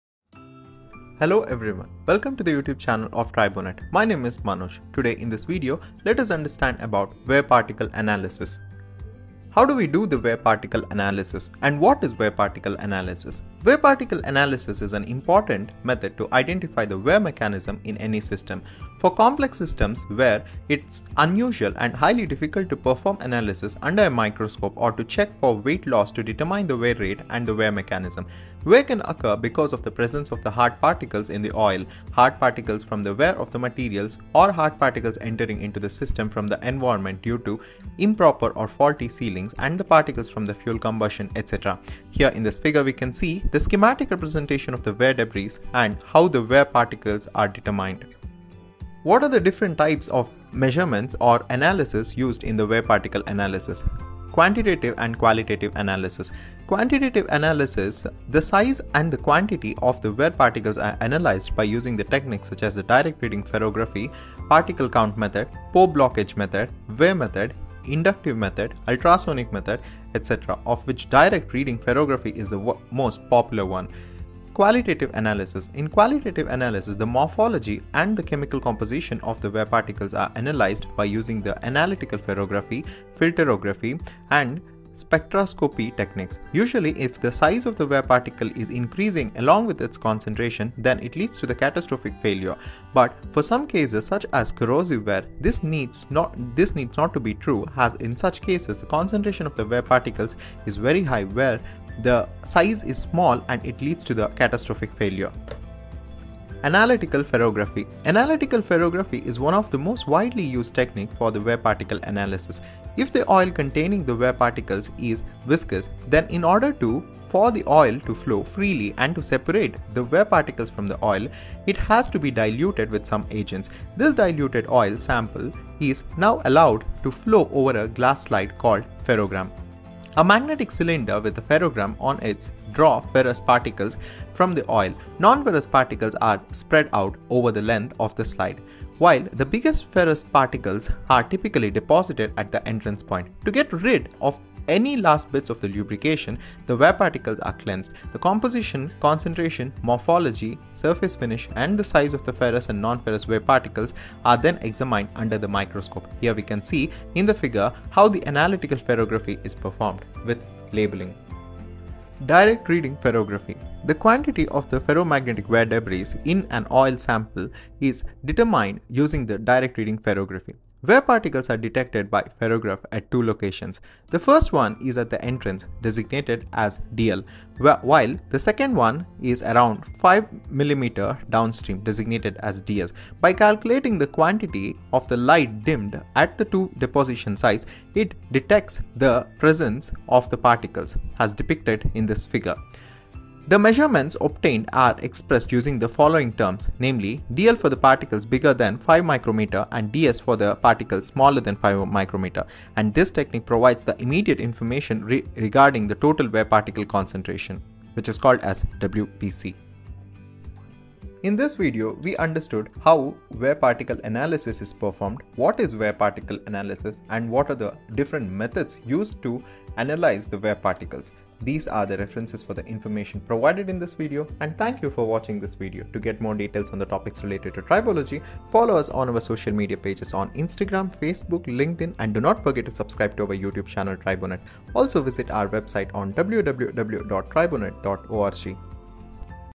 Wear particle analysis
In this video, the information on wear particle analysis is performed.

Topics covered:
1. What is wear particle analysis.
2. What are the types of methods used in this analysis.
3. Ferrography techniques.